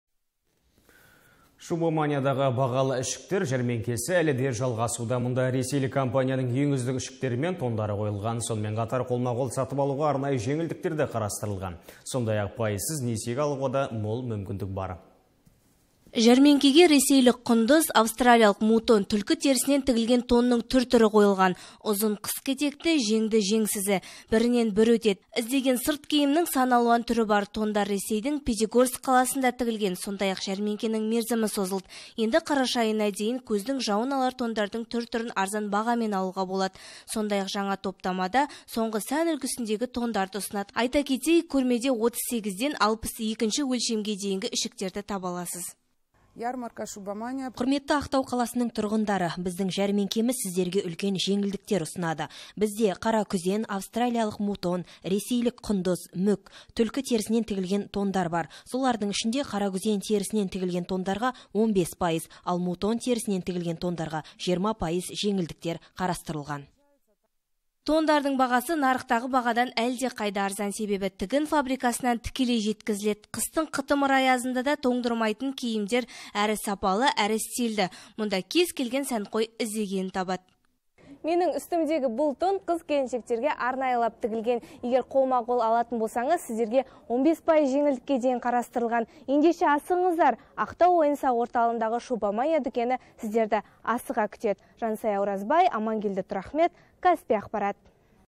"Шубаманиядағы" жеңілдіктер жалғасуда
"Шубаманиядағы" бағалы ішіктер жәрмеңкесі әлі де жалғасуда. Мұнда Ресейлік компанияның ең үздік ішіктері мен тондары қойылған. Сонымен қатар, қолма-қол сатып алуға арнайы жеңілдіктер де қарастырылған. Сондай-ақ, пайызсыз несиеге алуға да мол мүмкіндік бар.